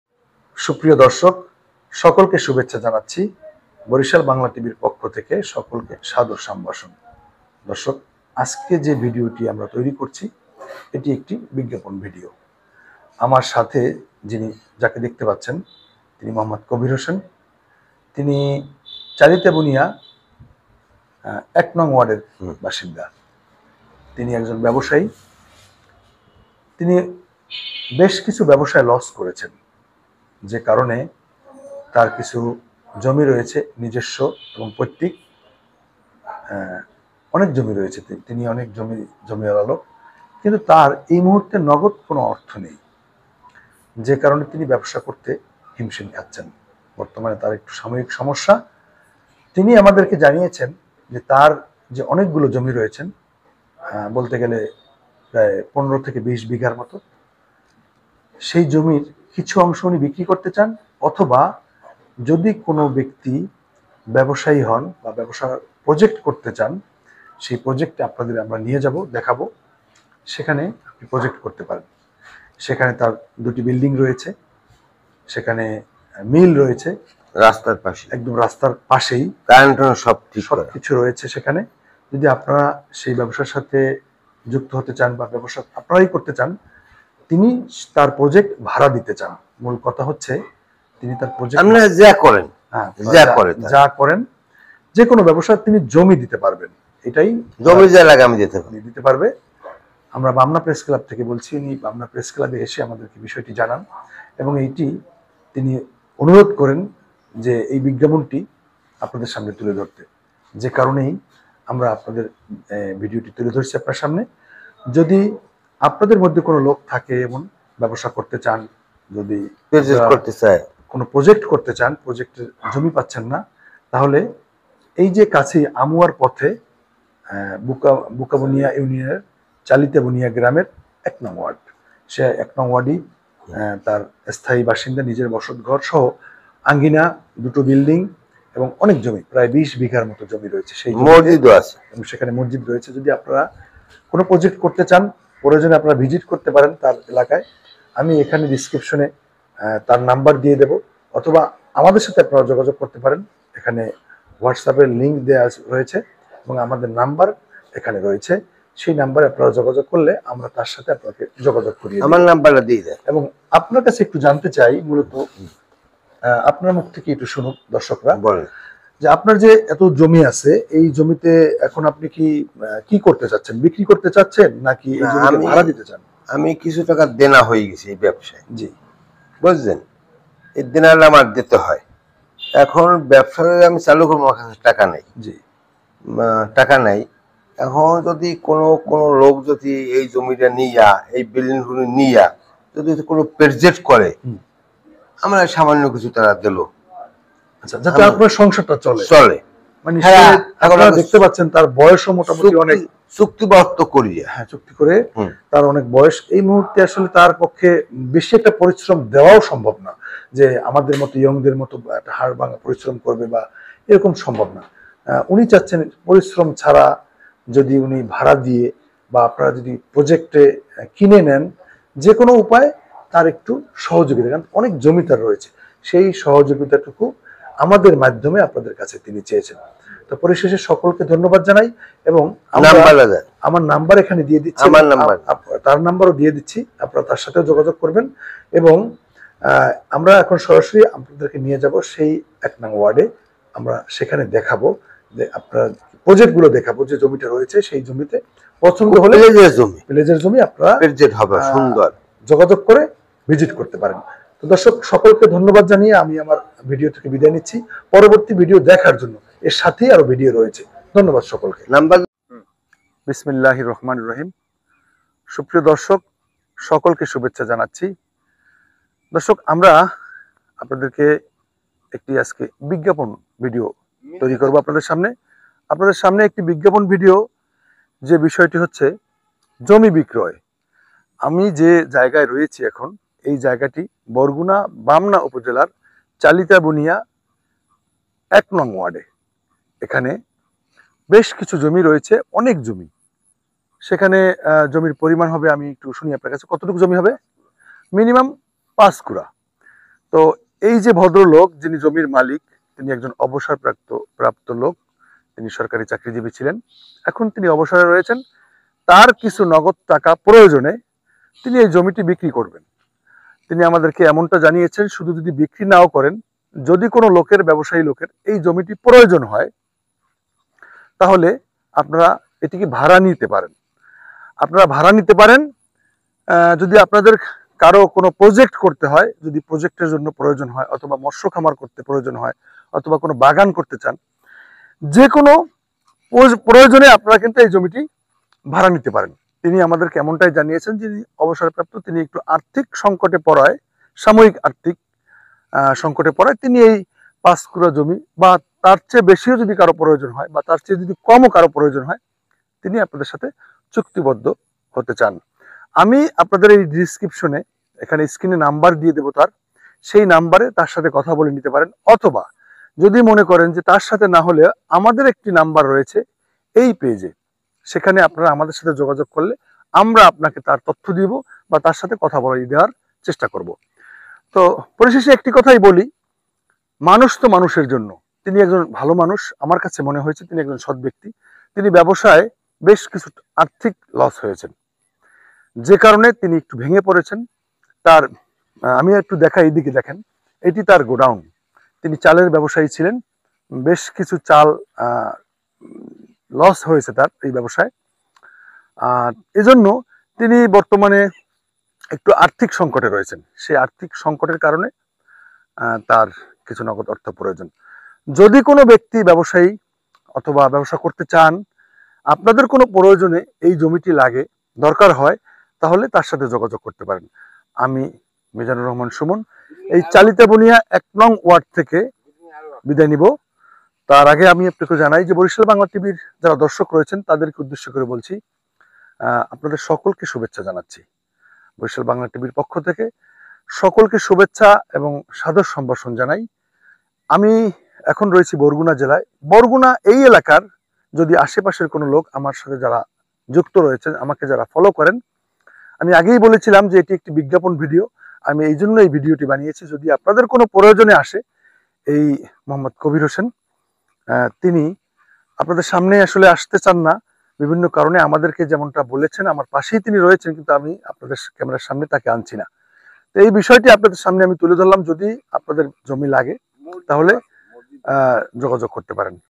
বরগুনার বামনা উপজেলার বুকাবুনিয়া ইউনিয়নের ১ নং ওয়ার্ডের মোঃ কবির হোসেন কিছু জমি বিক্রয় করতে চান।
একটি বিজ্ঞাপন ভিডিও, বরগুনার বামনা উপজেলার বুকাবুনিয়া ইউনিয়নের ১ নং ওয়ার্ডের মোঃ কবির হোসেন কিছু জমি বিক্রয় করতে চান।